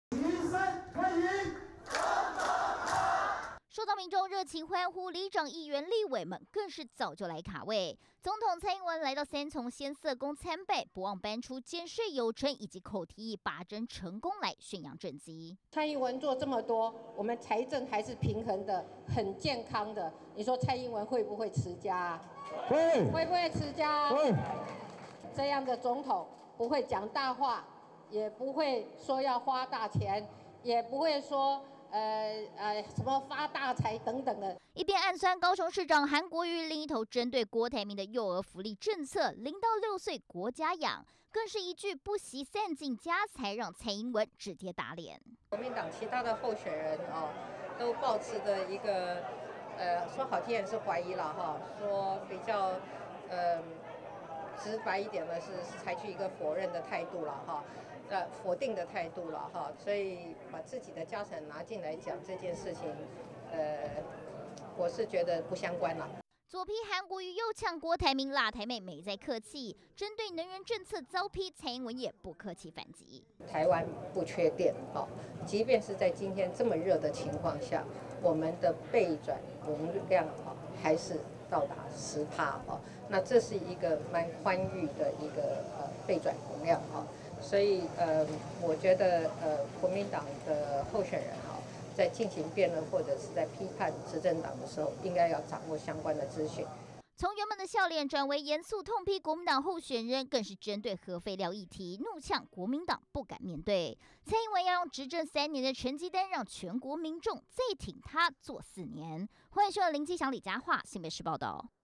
先嗇宮參拜宣揚政績 蔡英文暗酸韓發大財｜寰宇新聞 20190704
【寰宇新聞】總統蔡英文今天(4日)上午，前往新北市三重區先嗇宮參拜，不只宣揚三年來的執政成績，更是暗酸高雄市長韓國瑜，只會喊發大財。另外也針對國民黨初選參選人郭台銘，喊出不惜散盡家財，也要力推0到6歲國家養的育兒政策，對此，蔡英文直接打臉，強調國家大政與個人家產無關。